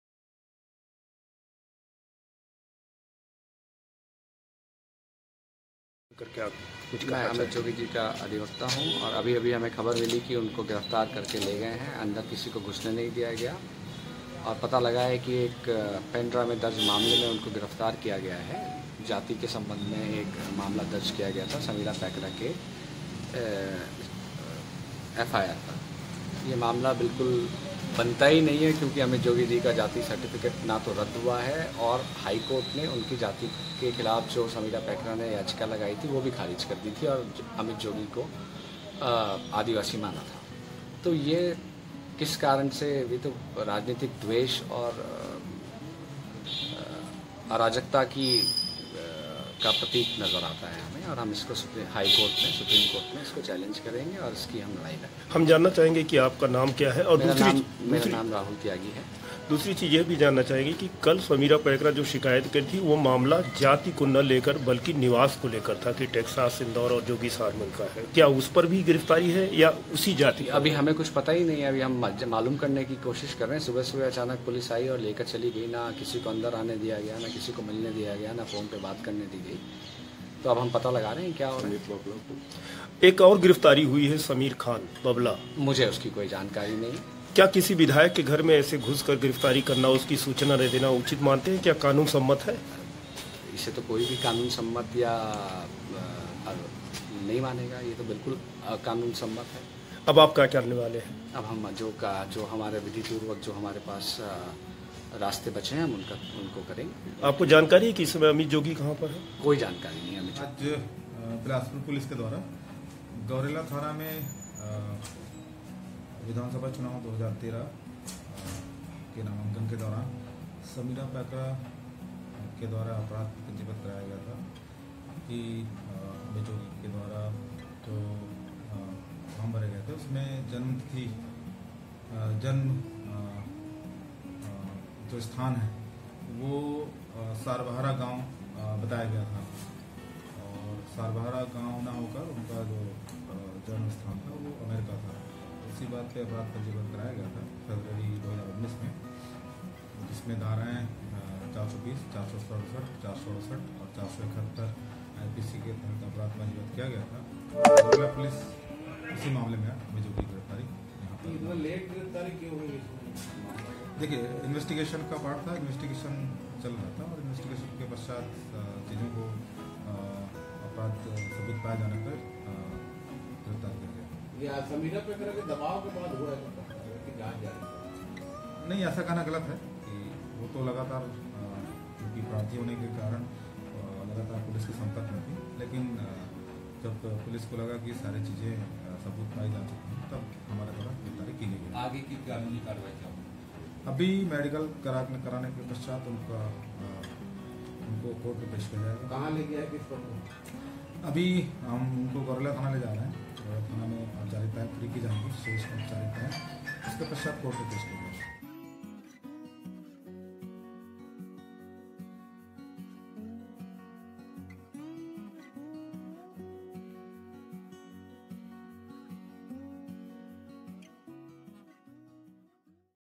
Amit Jogi के वकील ने कहा- इस तरह से गिरफ्तार करना गलत
अमित जोगी को गिरफ्तारी के बाद गौरेला ले जाया जा रहा है ।जहां थाने में उनके खिलाफ प्रमाण पत्र को लेकर मामला दर्ज है। उनके खिलाफ नागरिकता के संबंध में गलत जानकारी देने का आरोप लगाया गया है। खबर मिली है कि जिस समय अमित जोगी की गिरफ्तारी की गई गौरेला के एडिशनल एसपी भी मौजूद थे । सूत्रों ने बताया है कि उन्हें गिरफ्तारी के बाद गौरेला कोर्ट में पेश किया जा सकता है ।
NYOOOZ MP- Chhattisgarh डिजिटल दुनिया में एक अलग कलेवर और अंदाज़ के साथ मध्य प्रदेश और छत्तीसगढ़ के सभी शहरों की ख़बरों, जैसे